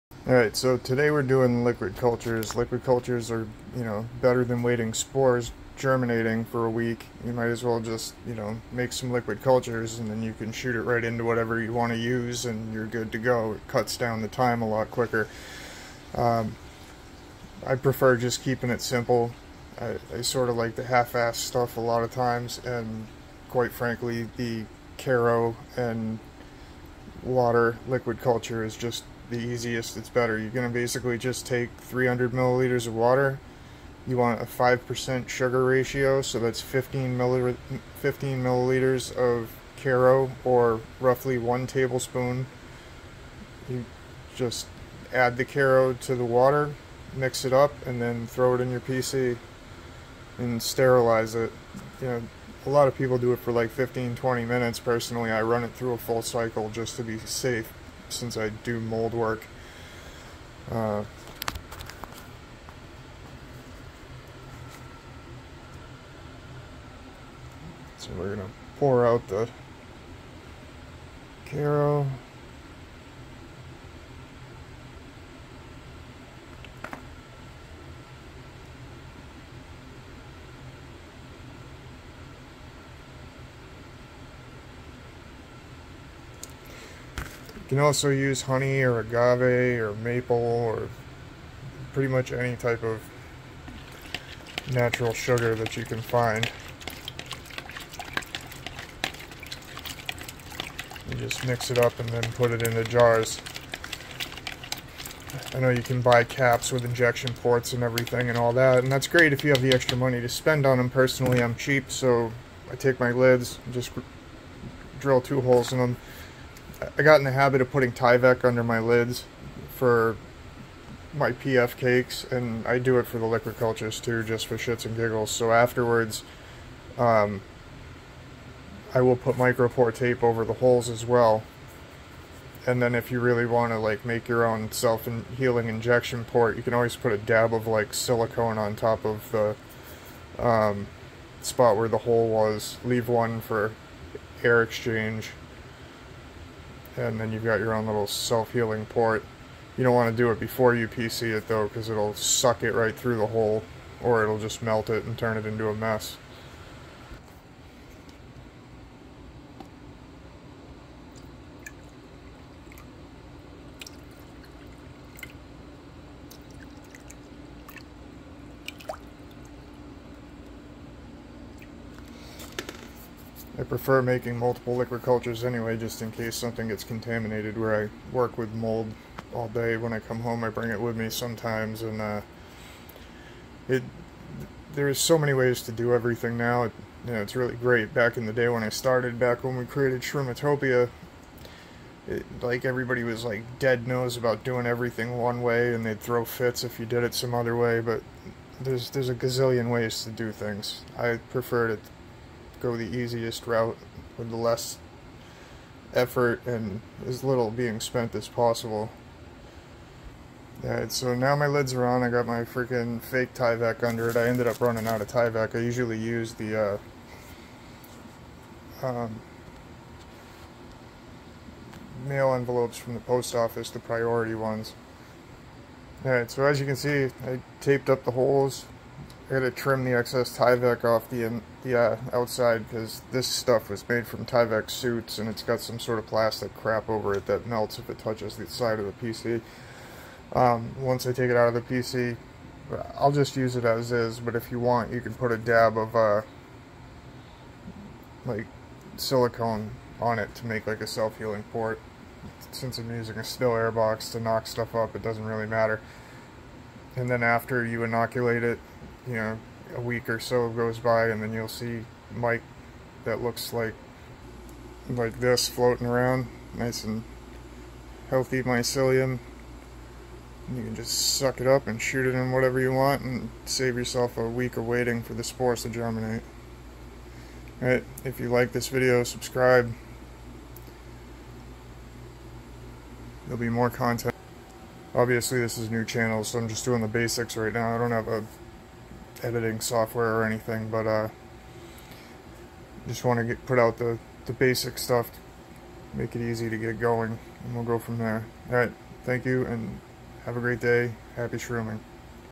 How to make an easy Liquid Culture (LC) 5% Corn Syrup with NHMI of Shroomotopia
This video shows a quick way to make liquid cultures, cheap.

We are just starting a channel,  so please bear with us...